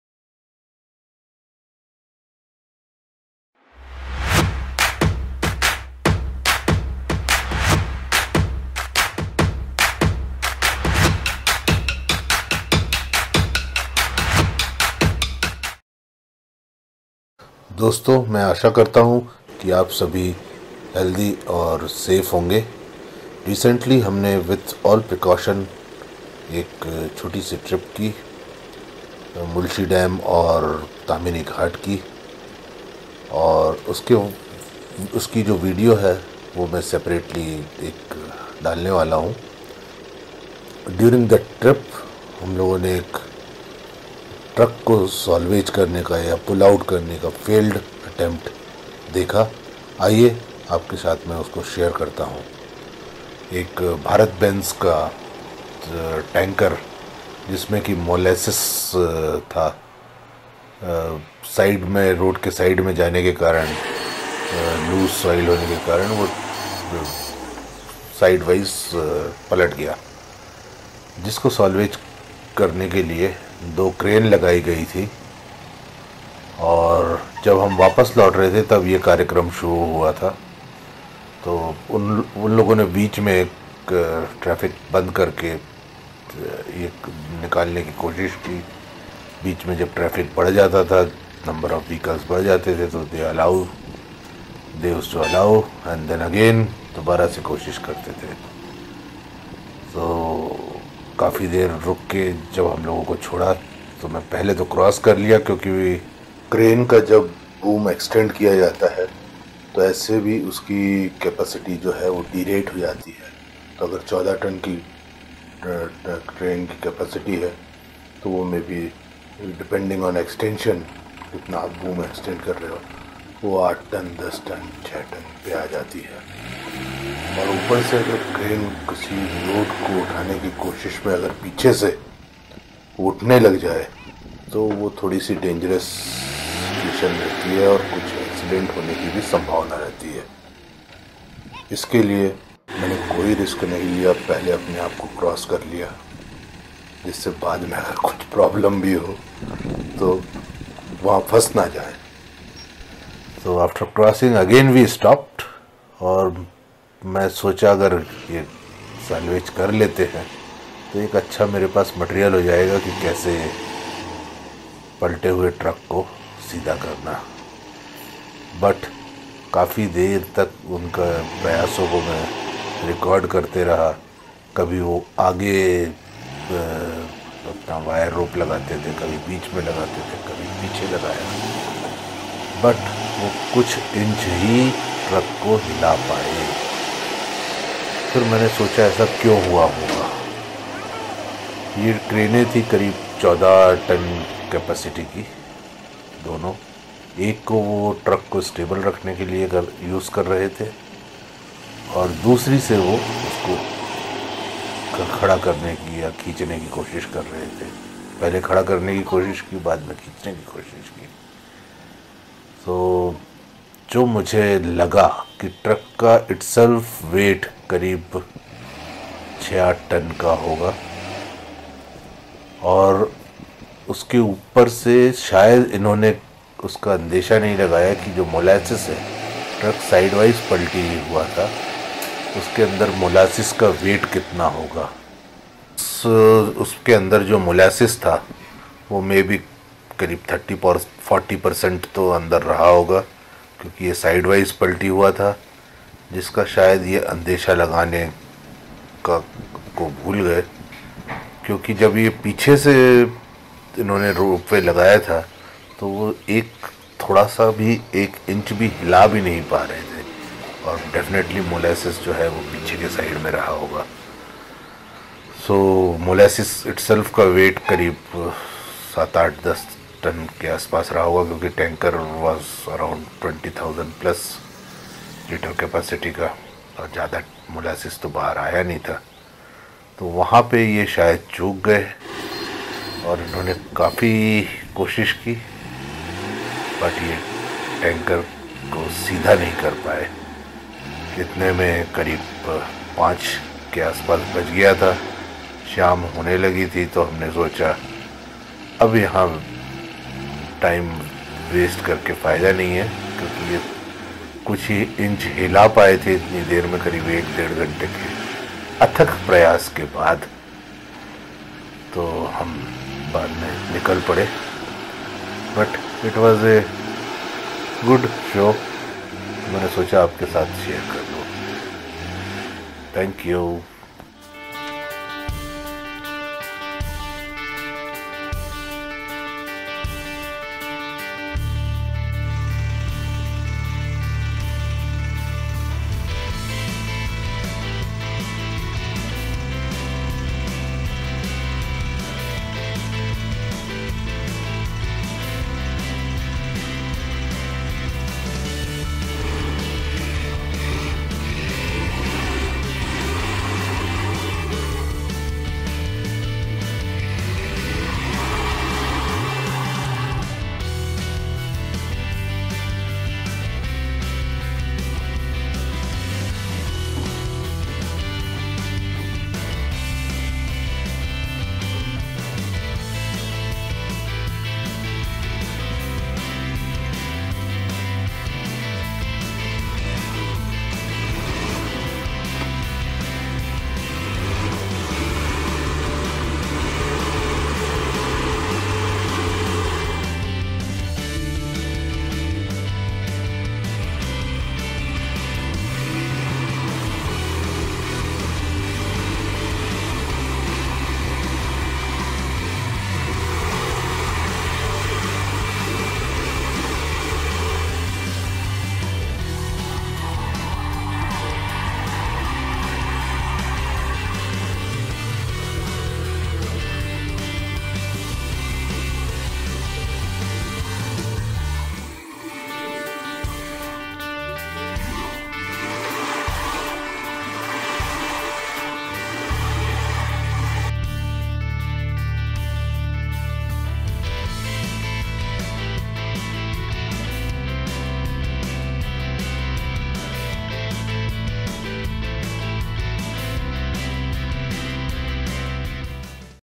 Places Around Pune : Unlock 1.0 : Attempts To Pullout a Truck @Mulshi Dam
V - 54
On our first trip after lockdown is over i. e. Unlock 1.0, we went on a day trip to Mulshi dam and Tamhini ghat, There we came across this incident of truck turned side wise in loose soil and all attempt to take it out failed.

Thanks,